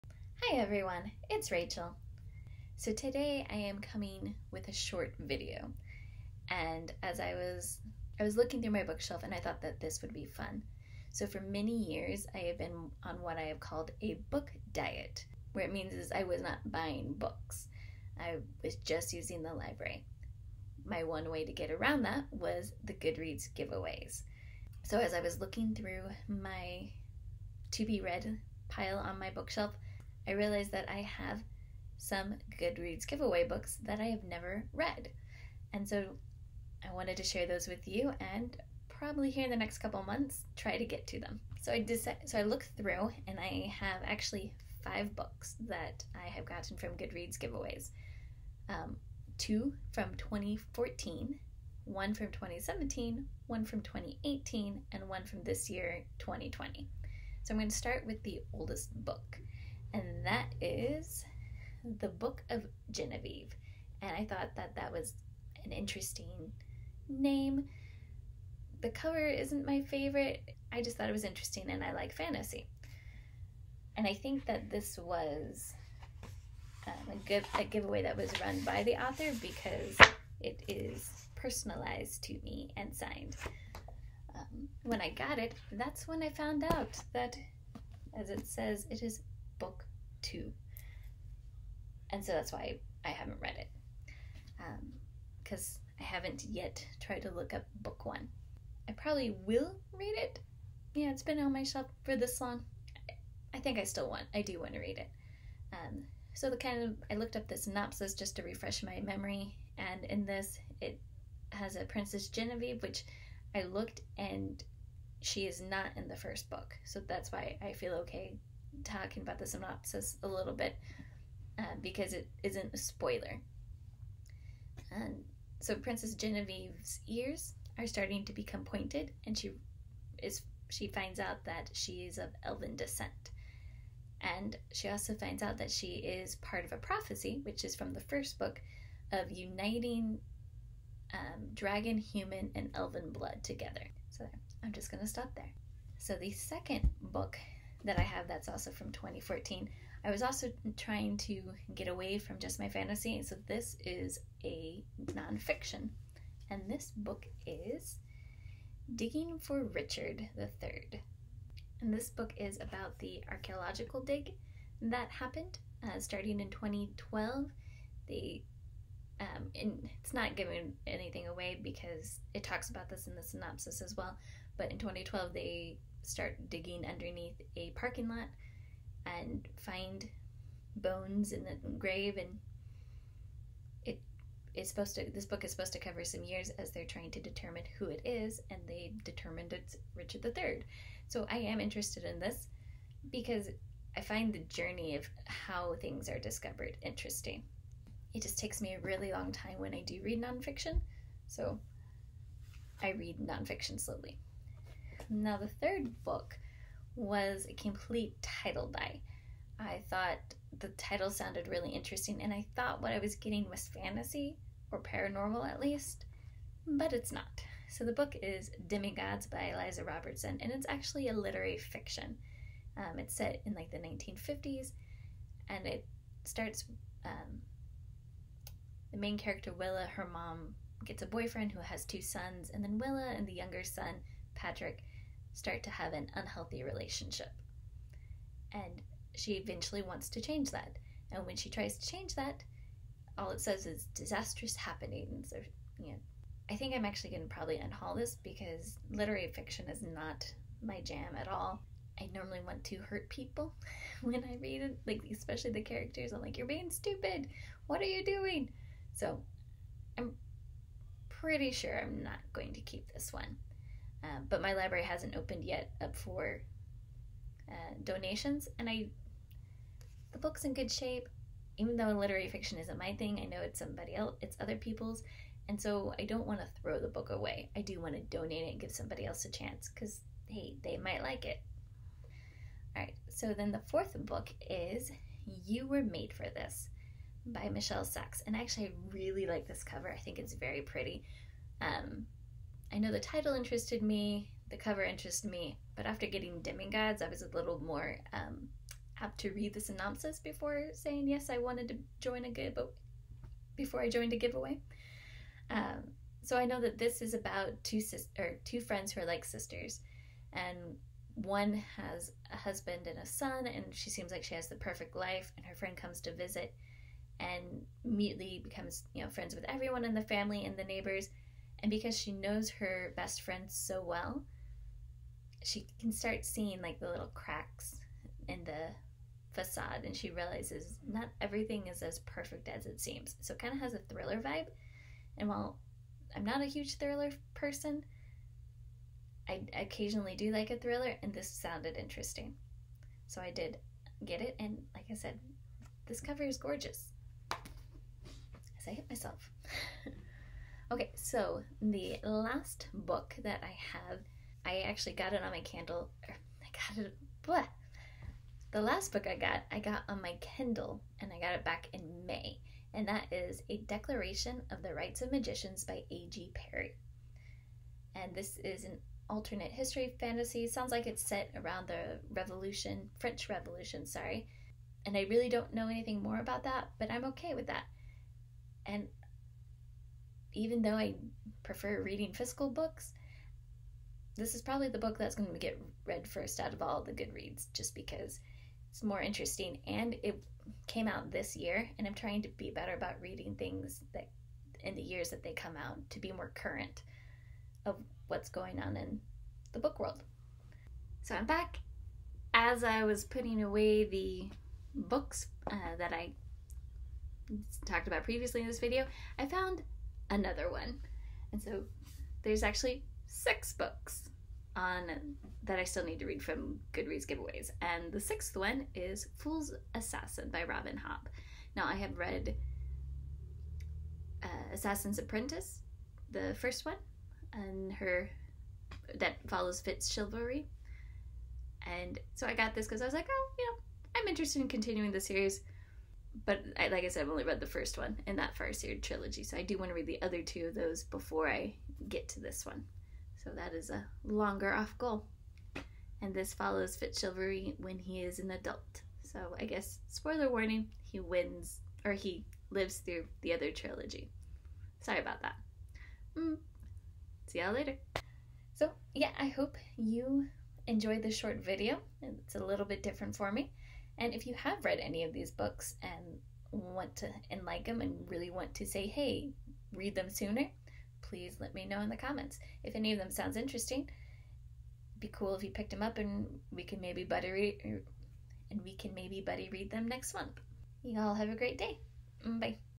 Giveaway Books To Read
Books I have received from Giveaways that I still need to read.